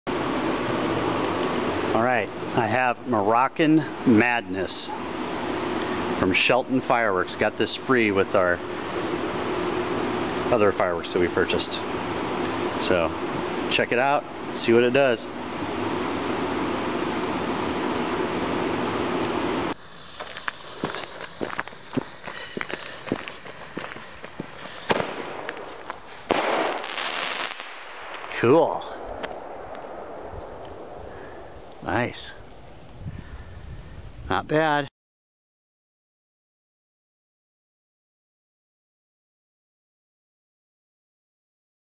Moroccan Madness - Shelton Fireworks
This a small single shot with break from Shelton Fireworks. It's not too bad for what it is.